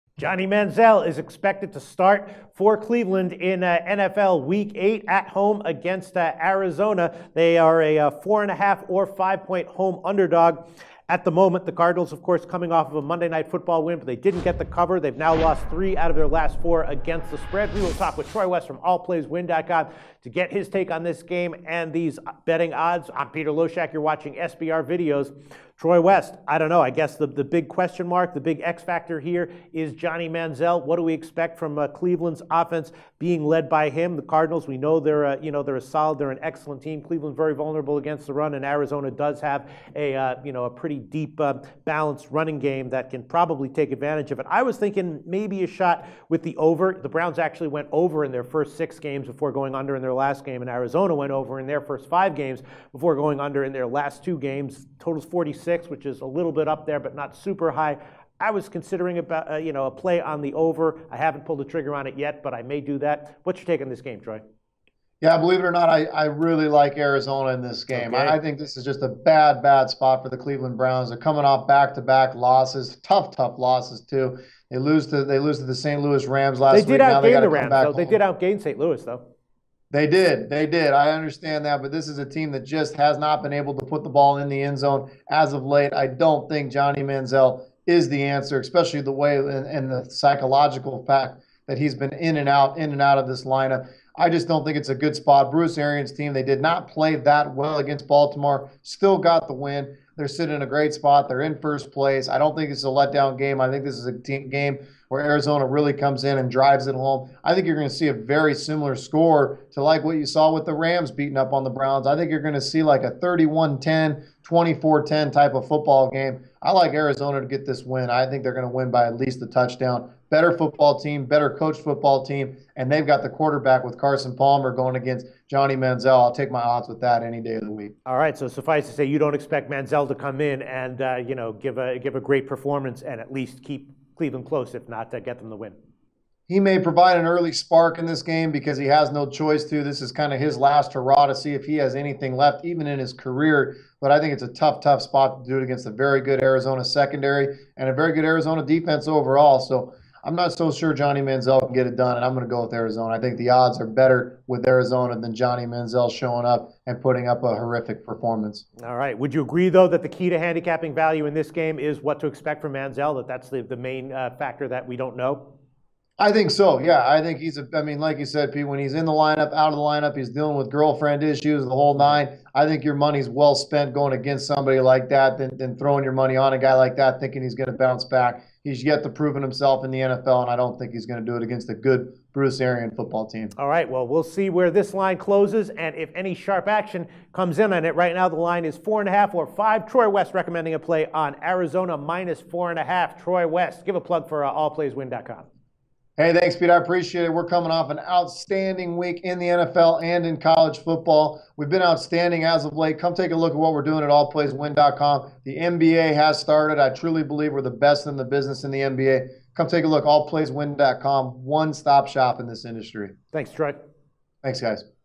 Cardinals vs Browns Preview: NFL Week 8 Picks
The Cleveland Browns will be a home underdog in NFL Week 8 against the formidable Arizona Cardinals, and if they can pull off the upset, it will have to be with disappointing Johnny Manziel leading them at QB. Professional handicapper Troy West from AllPlaysWin.com and SBR Videos host Peter Loshak discuss possible betting value on the game with the current market odds.